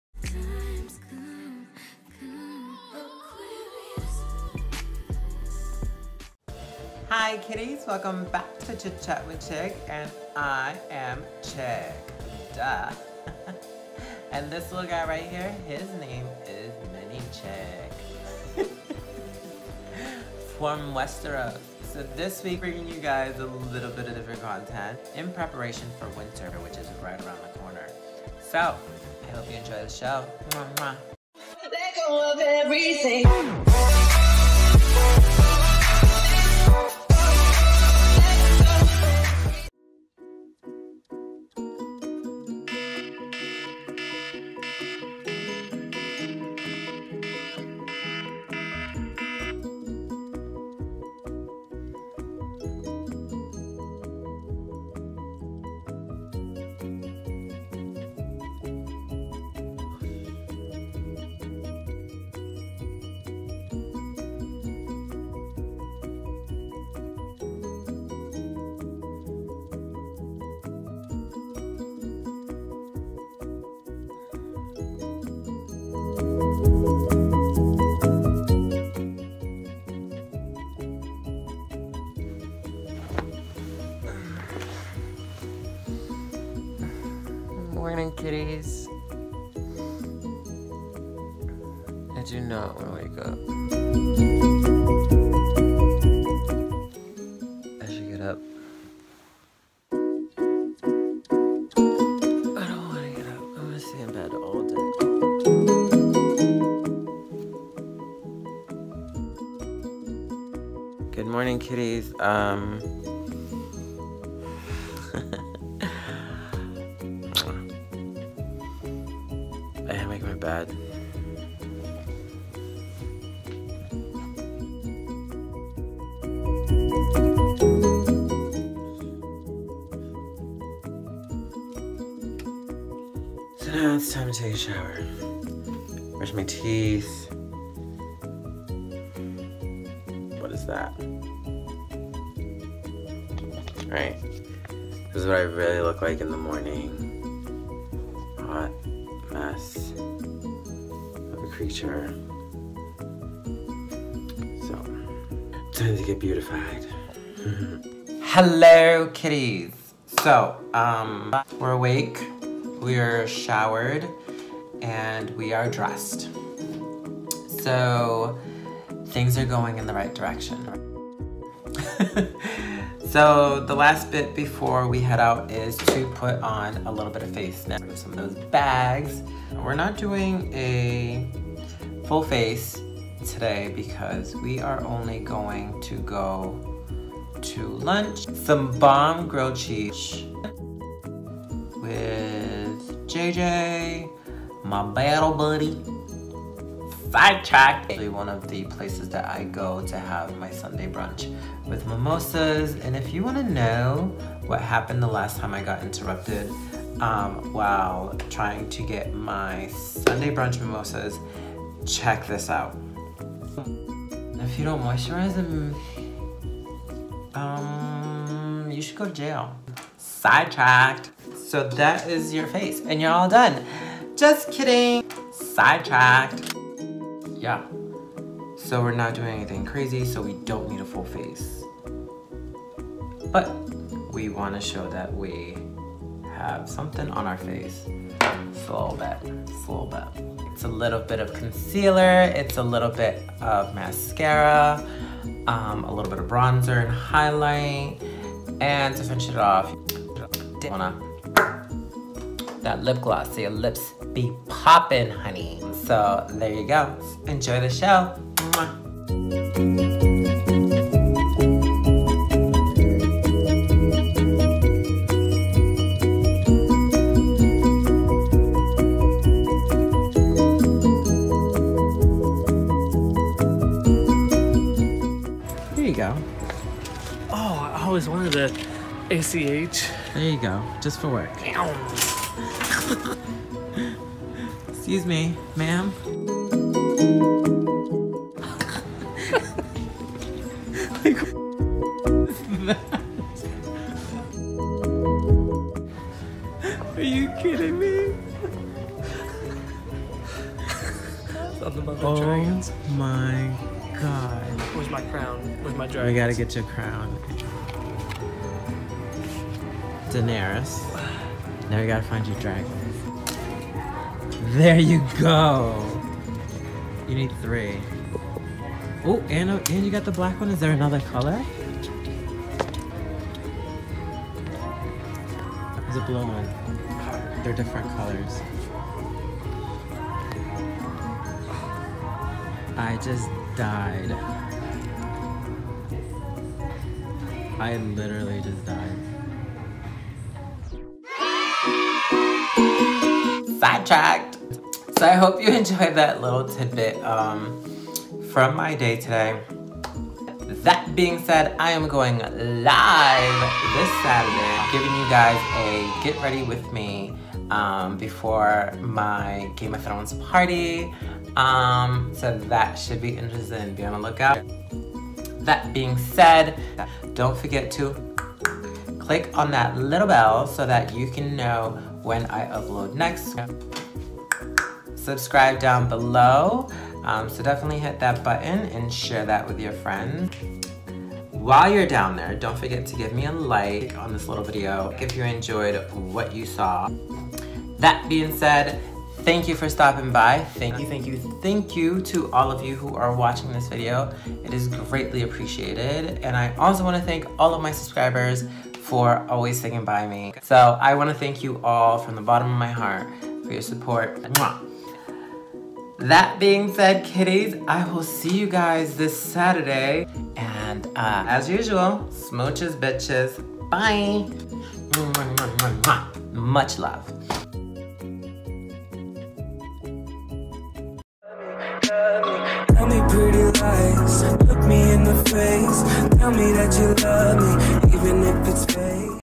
***GOT A NEW PET*** Ft. JJ Dude of Dragons
Smooches Beetches!!! Def a rough week but I am bringing you some waking REALNESS beetches cz ILYl!!! Did some GoT Party shoppin with the battle buddy introduced my new pet MiniChic lol PERO it is all for you kiddies ILYA smooches beetches!!!